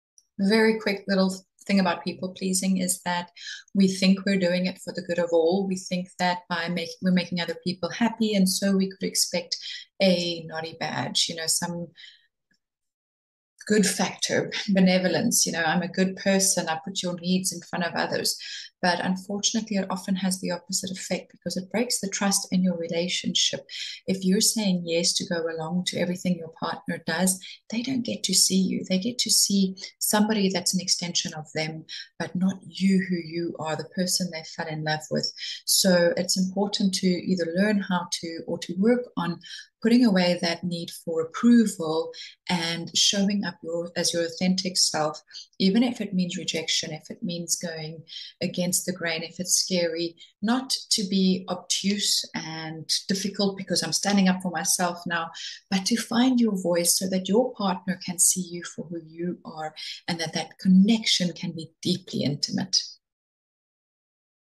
People pleasing
Are you a people pleaser?

Learn to find your voice so your partner can see who YOU are.

Join my free Facebook group where you can get more relationship tips.